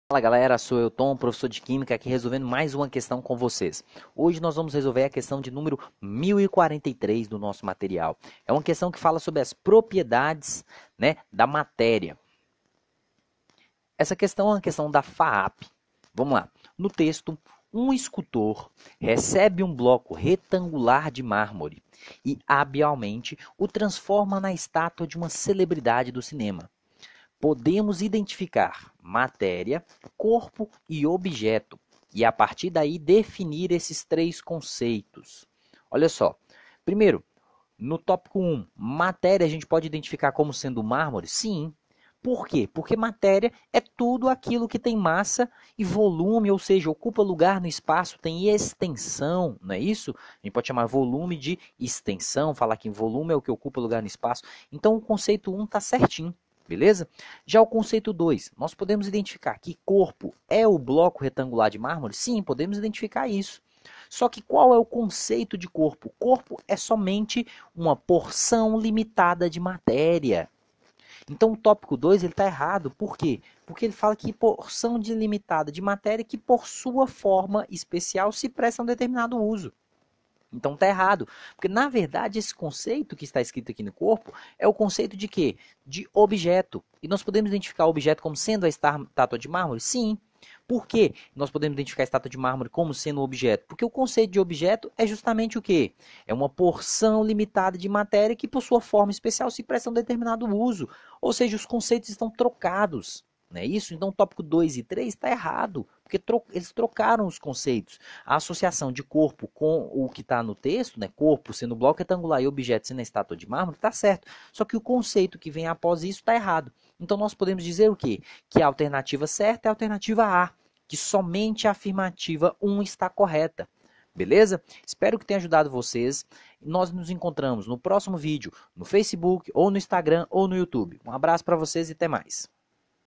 Questão 1043 - Matéria, corpo e objeto
1043 - (FAAP) No texto: “Um escultor recebe um bloco retangular de mármore e, habilmente, o transforma na estátua de uma celebridade do cinema”, podemos identificar matéria, corpo e objeto e, a partir daí, definir esses três conceitos.
I. Matéria (mármore): tudo aquilo que tem massa e ocupa lugar no espaço.
II. Corpo (bloco retangular de mármore):porção limitada de matéria que, por sua forma especial, se presta a um determinado uso.
III. Objeto (estátua de mármore): porção limitada de matéria.

Assinale:
a) se somente a afirmativa I é correta.
b) se somente a afirmativa II é correta.
c) se somente a afirmativa III é correta.
d) se somente as afirmativas I e II são corretas.
e) se as afirmativas I, II e III são corretas.